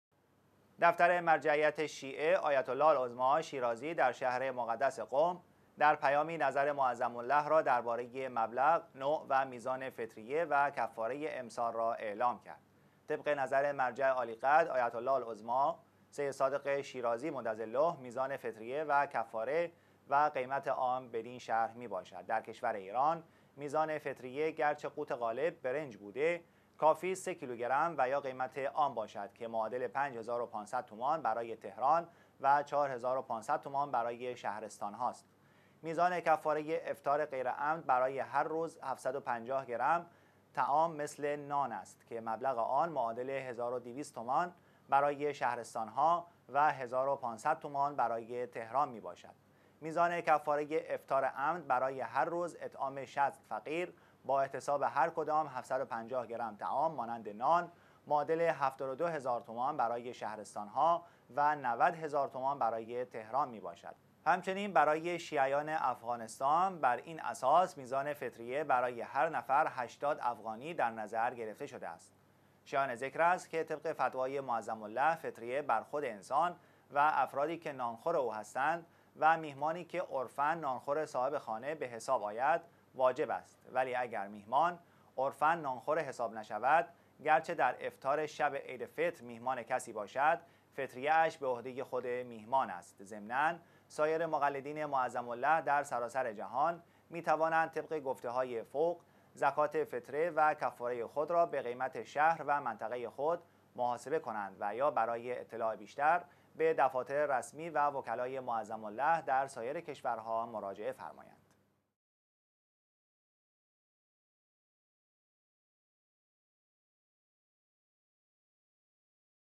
اعلام مبلغ زکات فطره، از سوي دفتر مرجع عالي قدر حضرت آيت الله العظمي سيد صادق حسيني شيرازي دام ظله
دفتر مرجعیت شيعه «آيت الله العظمي شيرازي» در شهر مقدس قم، در پيامي نظر معظم له را درباره ي مبلغ، نوع و ميزان فطریه و کفاره امسال، را اعلام کرد.

طبق نظر مرجع عالي قدر «آيت الله العظمي سيد صادق شيرازي» مدظله، ميزان فطريه و کفاره و قيمت آن بدين شرح است:

در کشور ایران، ميزان فطريه گرچه قوت غالب برنج بوده، کافي است سه کيلو گندم و يا قيمت آن باشد، که معادل 5500 تومان براي تهران، و 4500 تومان براي شهرستان ها است.

ميزان کفاره ي افطار غير عمد، براي هر روز «750 گرم» طعام مثل نان است که مبلغ آن معادل 1200 تومان براي شهرستان ها و 1500 تومان براي تهران مي باشد.

ميزان کفاره ي افطار عمد، براي هر روز اطعام شصت فقیر با احتساب هر کدام «750 گرم» طعام مانند نان، معادل 72.000 تومان براي شهرستان ها، و 90.000 تومان براي تهران مي باشد.

همچنين براي شیعیان افغانستان، بر اين اساس، ميزان فطريه براي هر نفر 80 افغاني در نظر گرفته شده است.

شايان ذکر است، که طبق فتواي معظم له، فطريه بر خود انسان و افرادي که نان خور او هستند و ميهماني که عرفاً نان خور صاحب خانه به حساب آيد، واجب است؛ ولي اگر ميهمان، عرفاً نان خور حساب نشود گرچه در افطار شب عيدفطر ميهمان کسي باشد، فطريه اش به عهده خود ميهمان است.

ضمناً ساير مقلدين معظم له در سراسر جهان، مي توانند طبق گفته هاي فوق، زکات فطره و کفاره ي خود را به قيمت شهر و منطقه ي خود، محاسبه کنند و يا براي اطلاع بيشتر، به دفاتر رسمي و وکلاي معظم له در ساير کشورها مراجعه فرمايند.